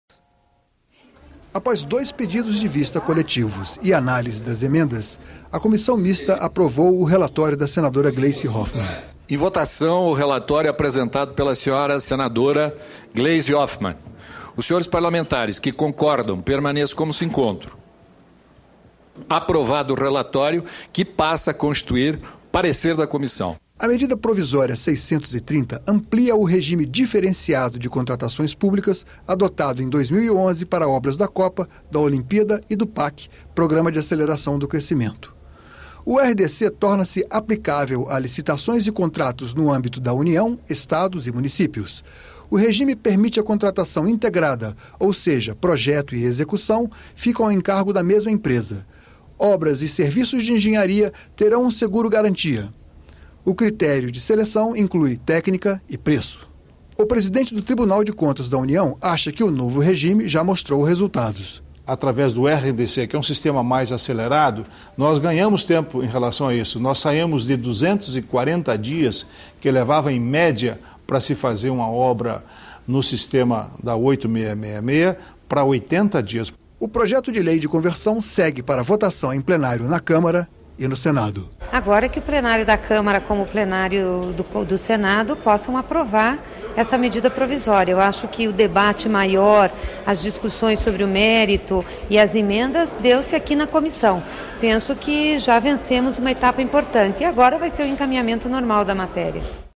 Jornalismo - Comissão mista aprova relatório à MP 630
A medida provisória que amplia o alcance do Regime Diferenciado de Contratações Públicas já pode ir aos Plenários da Câmara e depois do Senado. Pelo texto, todos os contratos e licitações de municípios, estados e União podem ter prazos mais curtos e procedimentos mais simples.